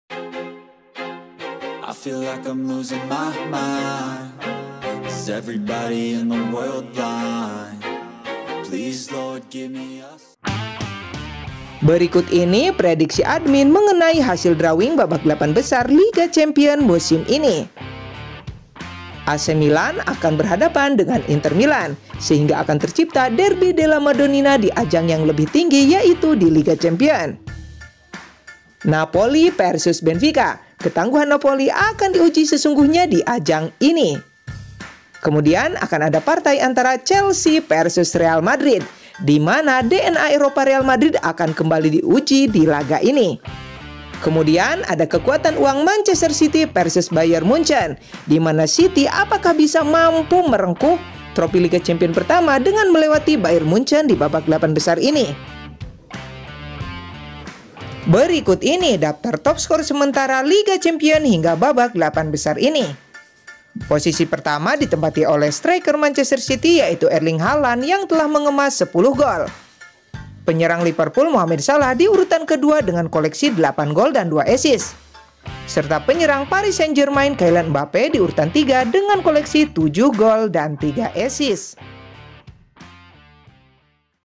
DRAWING 8 BESAR LIGA CHAMPION 2023 | Real Madrid vs Chelsea, AC Milan vs Inter,City vs Munchen
Drawing 8 besar liga Champion 2023 sesuai 8 tim yang lolos 8 besar liga Champions 2023 melihat hasil liga champion tadi malam admin memprediksi:

• AC Milan vs inter Milan
• Real Madrid vs Chelsea
• Napoli vs Benfica
• Bayern Munchen vs Manchester City

Top skor liga Champions 2023 sampai babak 8 besar dipimpin oleh Erling Haaland dengan koleksi 10 gol